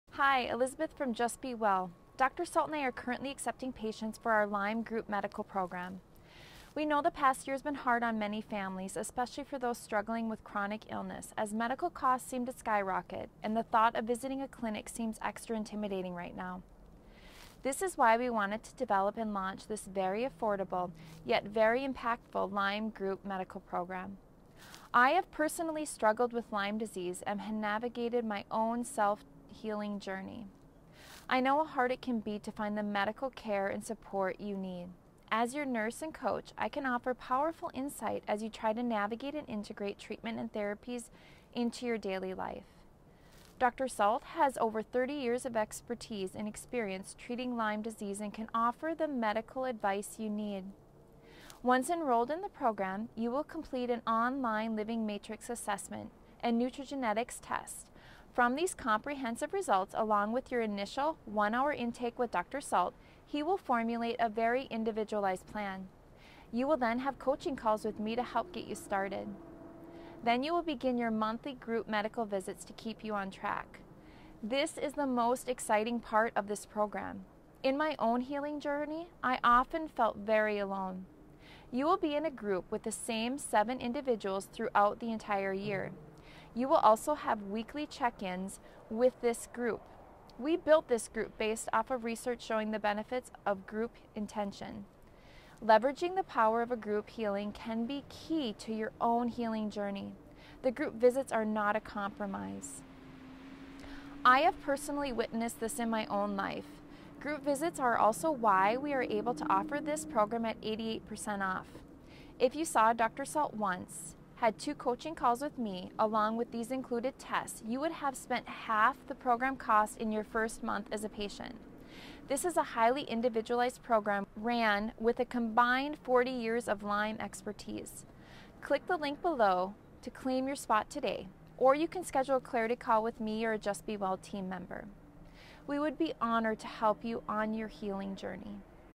What Just Be Well's Lyme Group Medical Program is All About
Why join Just Be Well's Lyme Group Medical Program?
{Valued at $24,289}

Research shows focusing healing on others unleashes innate healing potential for each individual as well.

That's one of the reasons we are excited to offer this group medical program.

Healing and managing chronic disease is a journey — and it can feel lonely. This group model builds community, comradery and accountability for transformation.

-Elizabeth Rosemeier, Just Be Well RN and Wellness Coach